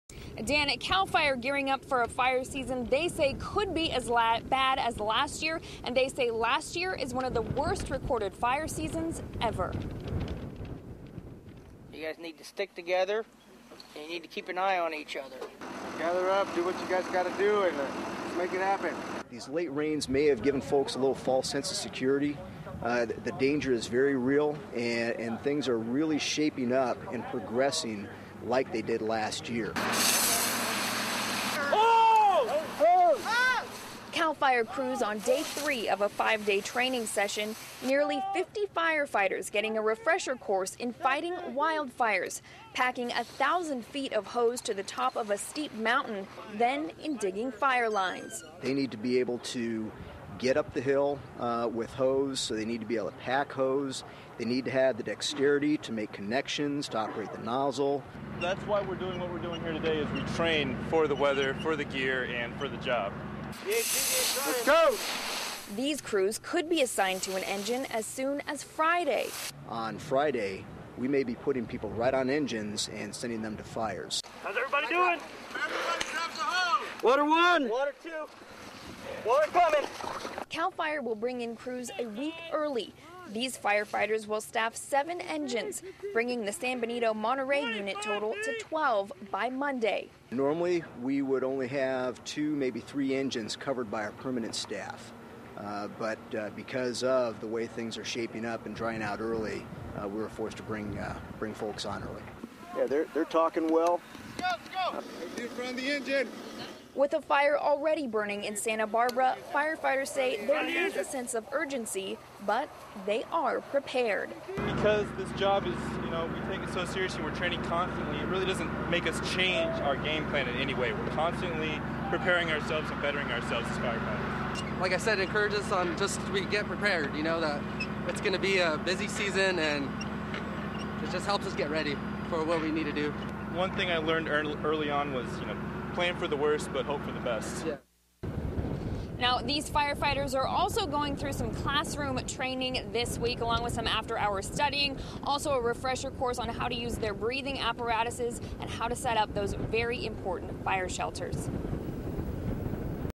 Local Firefighters Gear Up For Fire Season
Local firefighters are getting ready for the fire season in King City.